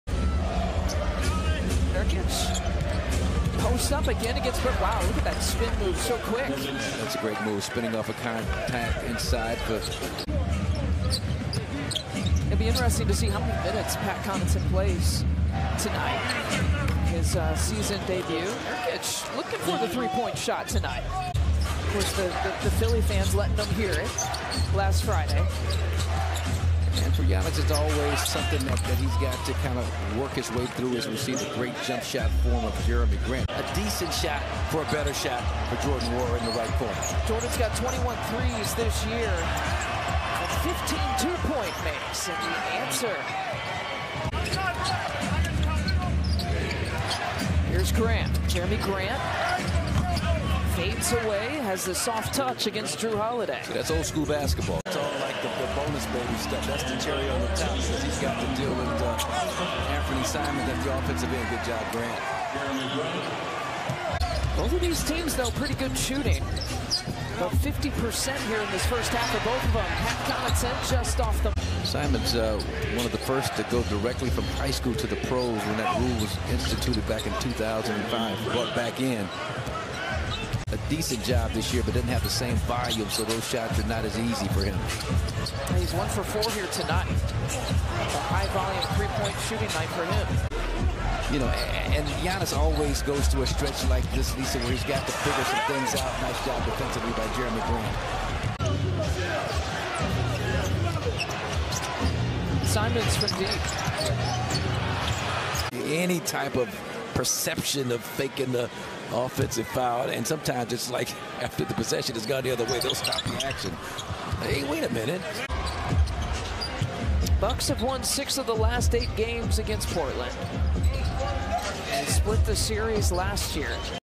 Jerami Grant vs Milwaukee Bucks | Nov 21, 2022
18 PTS
4 REB
2 AST

Jerami Grant vs Milwaukee Bucks
Portland Trail Blazers vs Milwaukee Bucks